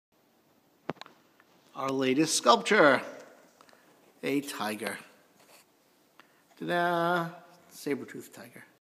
sabertooth tiger sculpture at REB research
Saber tooth tiger sculpture- the eyes and face follow you!!!